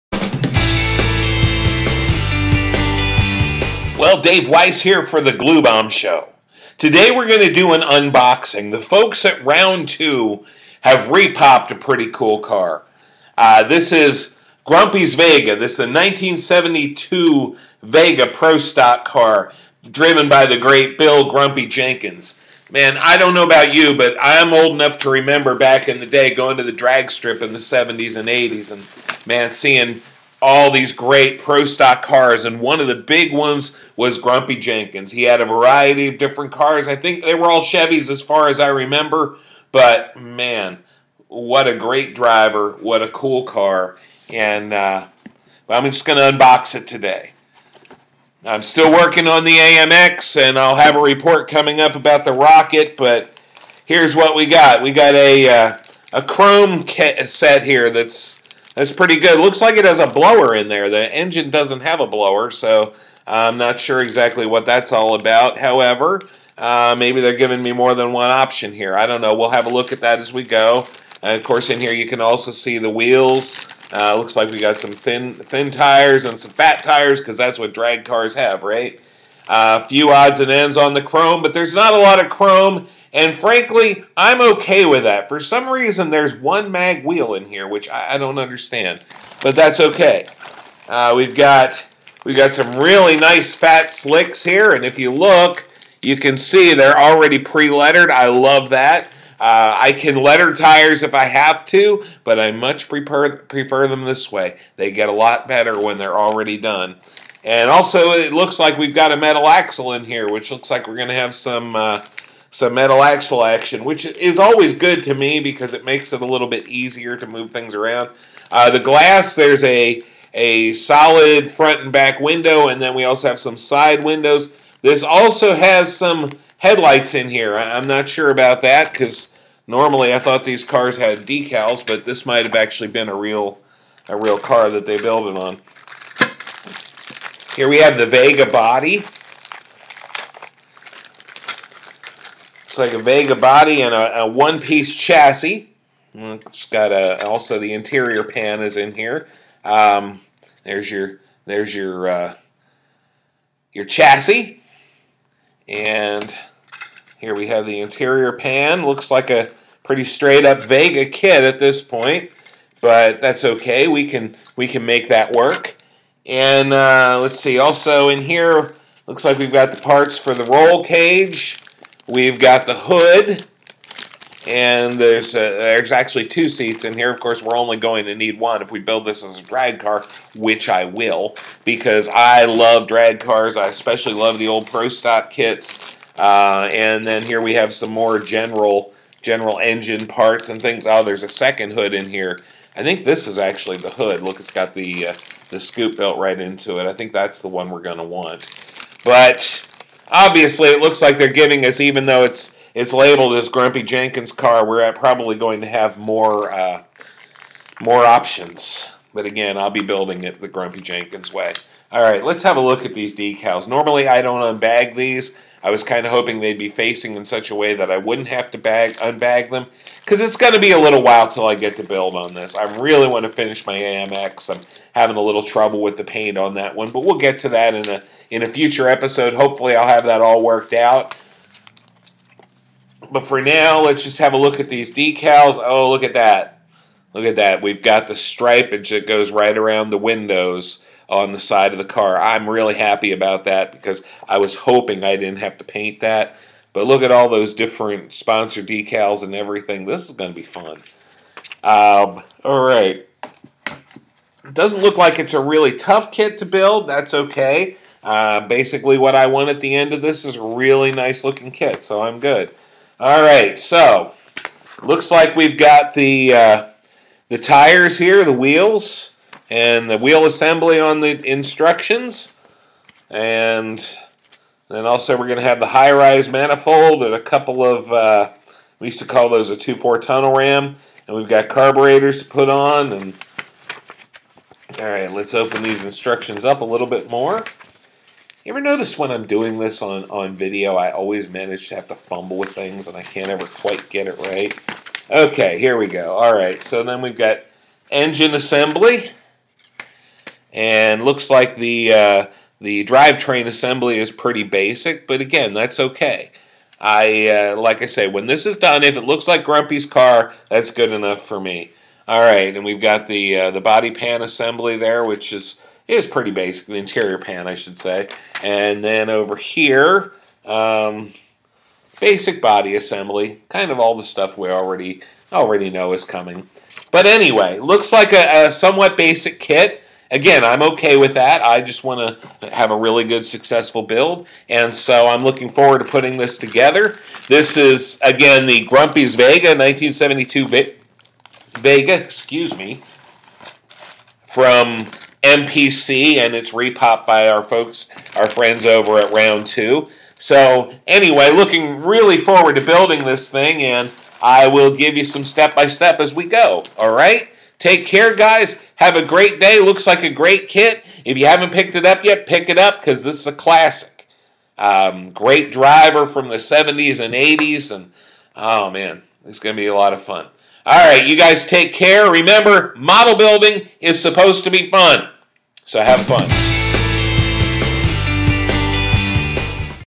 The Glue Bomb Show Episode 35: Unboxing Grumpy's Vega
Our friends at Round Two Models have re-popped Bill Grumpy Jenkins' classic 1972 Vega Pro-Stocker. It's the next kit to go on the table here at the Glue Bomb Show, so I thought I would unbox it and give you a preview. Looks like fun, which is good because as you know, Model Building is Supposed to be fun!